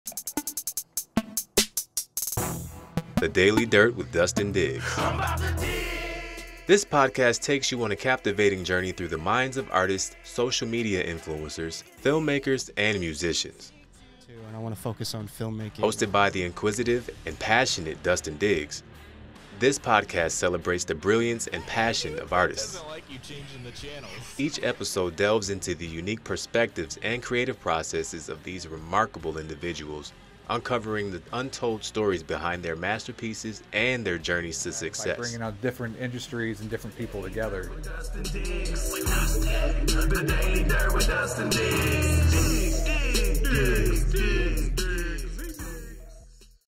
What is the Daily Dirt with Dustin Diggs?
The Daily Dirt with Dustin Diggs

Unearth the stories of creativity and inspiration with "The Daily Dirt with Dustin Diggs," the podcast that takes you on a captivating journey through the minds of artists, social media influencers, filmmakers, and musicians.

About the Podcast:

The Daily Dirt with Dustin Diggs" is your daily dose of artistic exploration and discovery. Hosted by the inquisitive and passionate Dustin Diggs, this podcast celebrates the brilliance and passion of artists, influencers, filmmakers, and musicians from around the globe. Each episode delves into the unique perspectives and creative processes of these remarkable individuals, uncovering the untold stories behind their masterpieces and their journey to success.

Meet Dustin Diggs:

Dustin Diggs is an artist, seasoned storyteller, and a keen observer of the creative world. With a background in arts journalism and a deep appreciation for various forms of artistic expression, Dustin brings a genuine and empathetic approach to each interview. His innate ability to connect with guests allows for intimate conversations that reveal the true essence of their creativity and the driving forces behind their artistic endeavors.

What to Expect:

With "The Daily Dirt with Dustin Diggs," you'll embark on a daily adventure into the world of creativity and artistry. Each episode features in-depth interviews with artists, social media influencers, filmmakers, and musicians, offering an intimate look into their lives and creative processes. From celebrated names to rising stars, every guest brings a unique perspective, sharing insights into the inspirations that fuel their art and the challenges they've overcome on their creative journey. Join Dustin Diggs every day and be captivated by the compelling stories of artistic pursuit, the dedication to craft, and the profound impact of creativity on our lives. Tune in to "The Daily Dirt with Dustin Diggs" and let the magic of artistic expression ignite your own creative flame.